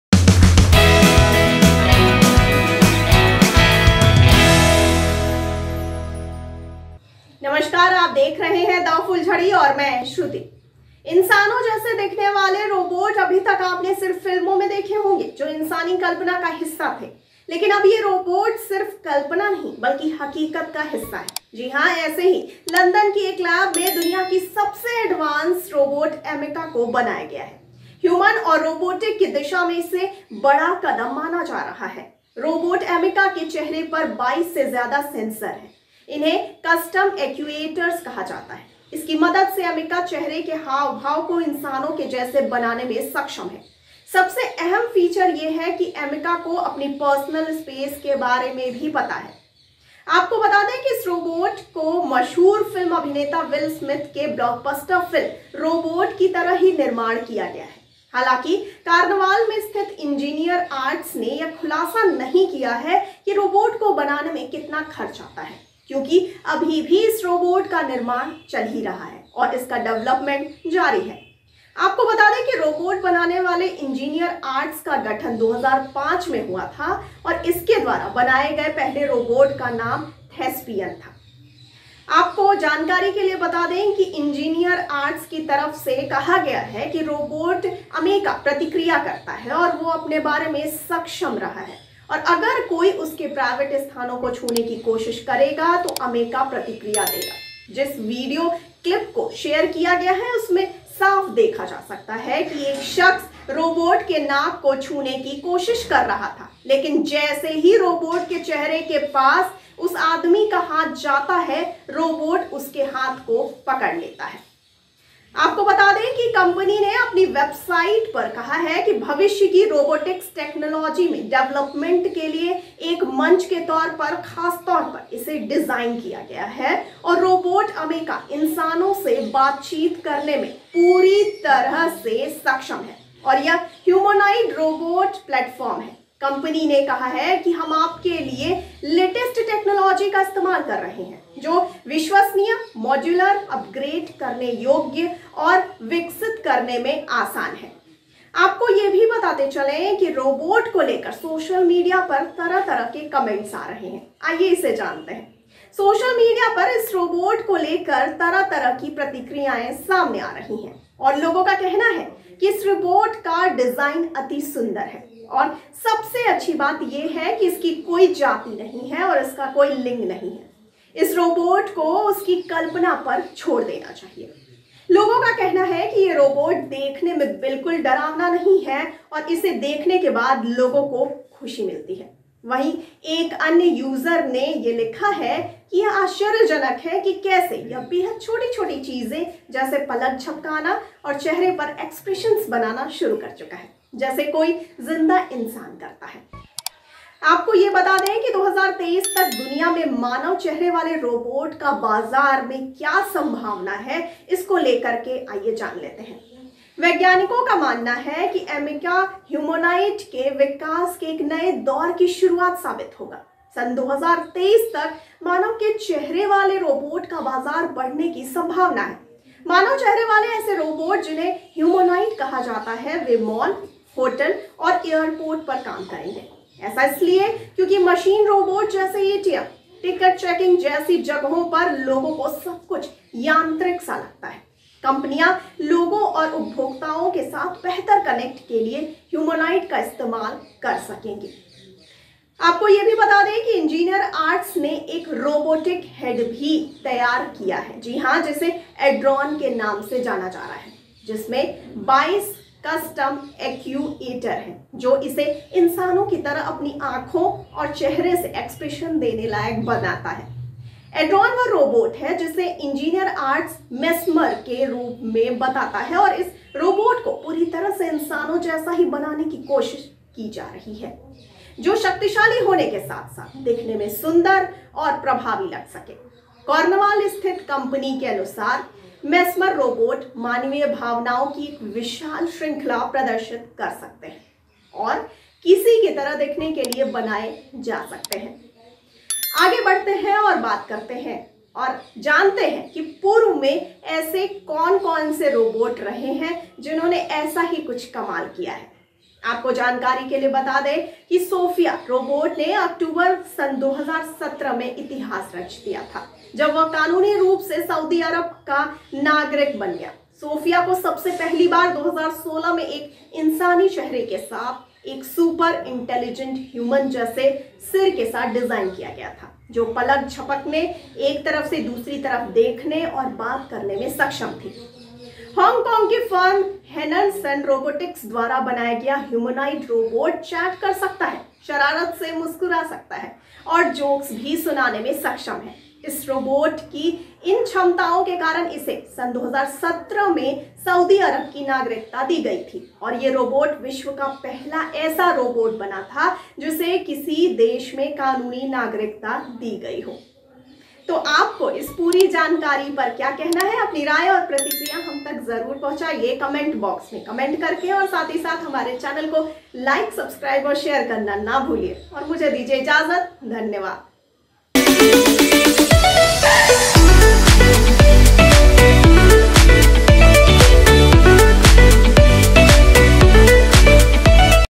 विश्व का सबसे एडवांस रोबोट लांच I चेहरे के एक्सप्रेशंस कमाल और रखती है अपनी पर्सनल स्पेस का ख्याल I
इंसानों जैसे दिखने वाले रोबोट अभी तक आपने सिर्फ फिल्मों में देखे होंगे, जो इंसानी कल्पना का हिस्सा थे। लेकिन अब ये रोबोट सिर्फ कल्पना नहीं बल्कि हकीकत का हिस्सा हैं। ऐसे ही लंदन की एक लैब में दुनिया की सबसे advance Robot Ameca को बनाया है। ह्यूमन और रोबोटिक की दिशा में इसे बड़ा कदम माना जा रहा है। Robot Ameca के चेहरे पर 22 से ज्यादा सेंसर हैं। इन्हें custom actuators कहा जाता है। इसकी मदद से एमिका चेहरे के हाव भाव को इंसानों के जैसे बनाने में सक्षम है। सबसे अहम फीचर है कि एमिका को अपनी पर्सनल स्पेस के बारे में पता है।